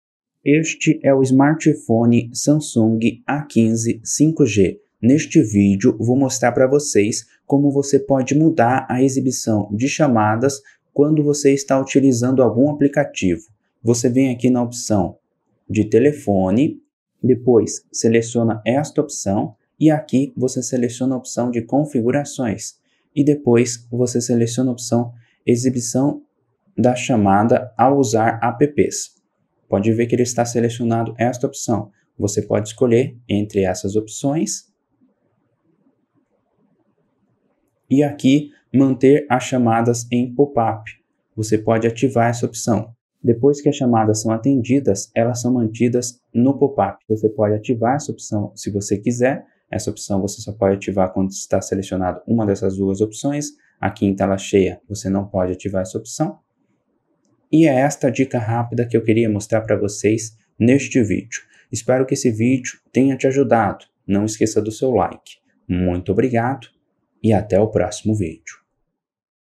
Samsung A15 5G Como mudar a exibição de chamadas quando você está usando um aplicativo
Neste vídeo vou te ensinar como mudar a exibição de chamadas quando você está usando um aplicativo no Samsung A15 5G